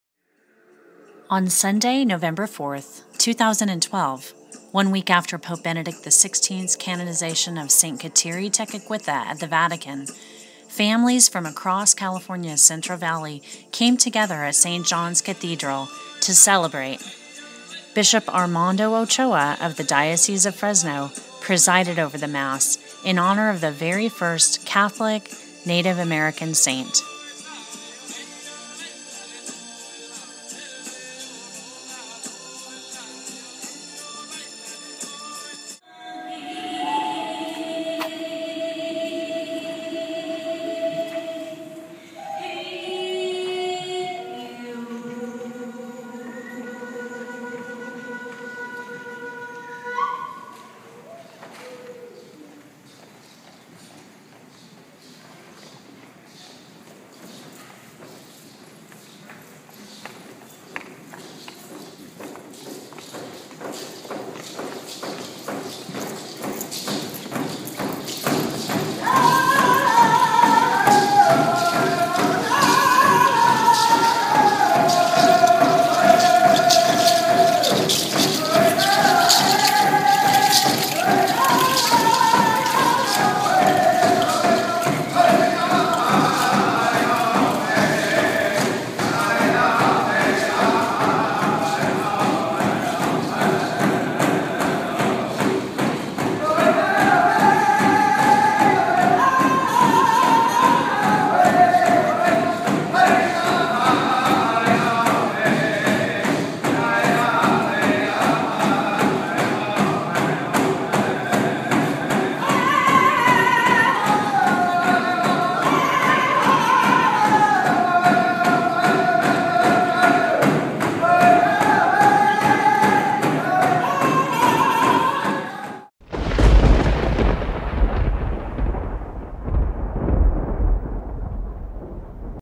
Saint Kateri Tekakwitha Mass of Celebration
On Sunday, November 4, 2012 the Diocese of Fresno celebrated a special Mass in honor of the canonization of Saint Kateri Tekakwitha. Bishop Armando Ochoa presided over the Mass at St. John's Cathedral in Fresno, California. Read the full story on this special Mass at read the full story